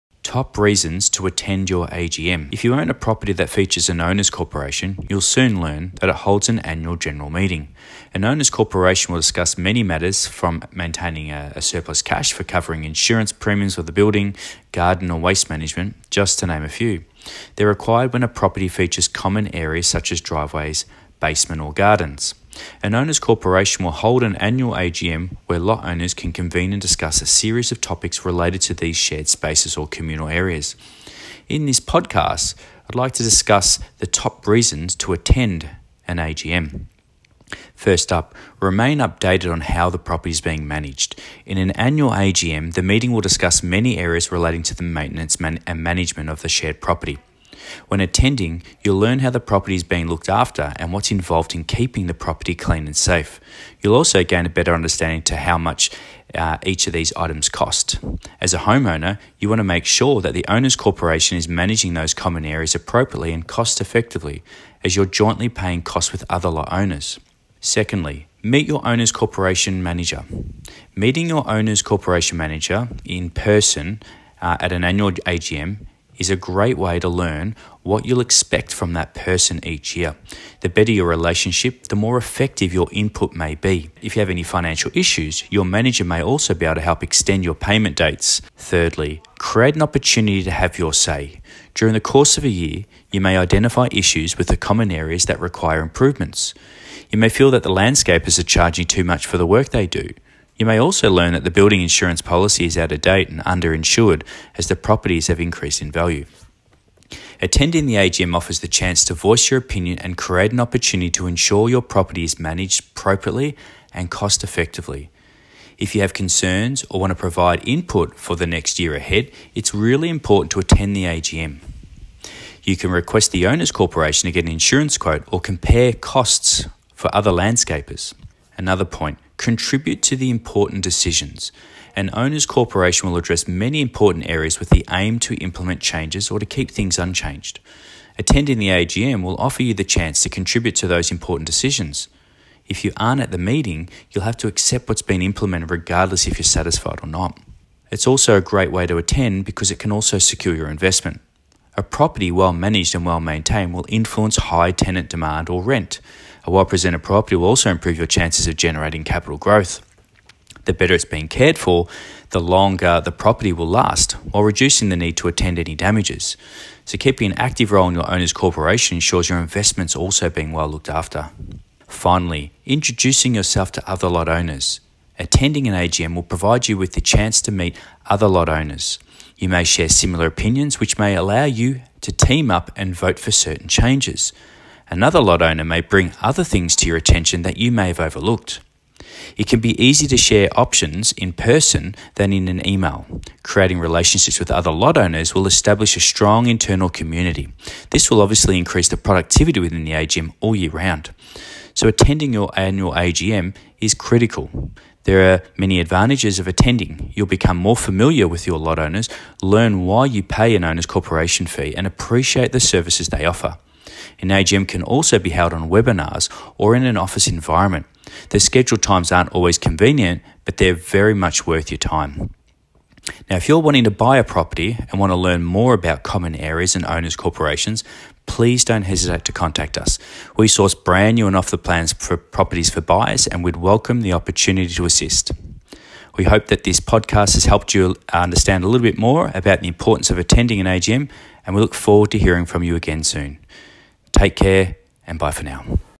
Top reasons to attend your AGM
An owners corporation will hold an annual AGM where lot owners convene and discuss a series of topics related to the shared spaces or common property. Here we discuss the top reasons to attend your AGM.